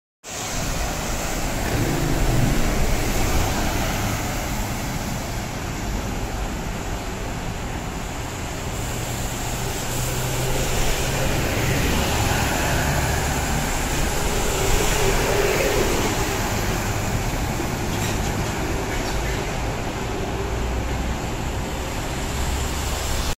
【港島東區之特快路線，只限平日早上繁忙時間服務】過海隧巴933線（用車：6479／WR5356）駛離西區海底隧道收費廣場（南行站）後，再經西區海底隧道（九龍入口）前往港島
過海巴士路線933，由城巴獨營，來往荃灣西站及西灣河，途經新界荃灣市中心、環宇海灣，以及港島北角、鰂魚涌、太古，回程另加停中環。

此路線只於星期一至五上、下午繁忙時間分別提供去程及回程服務，取道荃灣路、三號幹線及中環及灣仔繞道往返荃灣及港島東。